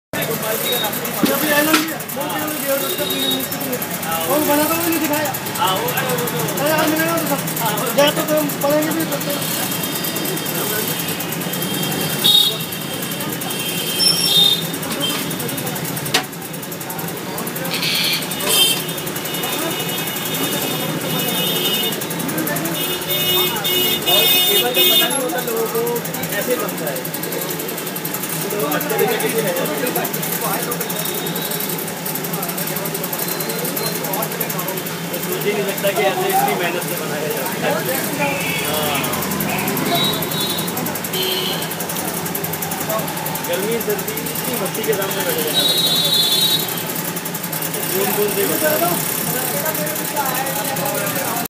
Jaipur Teej 2013, the making of Ghevar
Ghevar, is a special Rajasthani sweet, which is especially meant to be enjoyed during Teej and the month of Sawan. This video was made in Gopal ji ka Raasta, Jaipur, where these shops are in the business of making Ghevar through the year.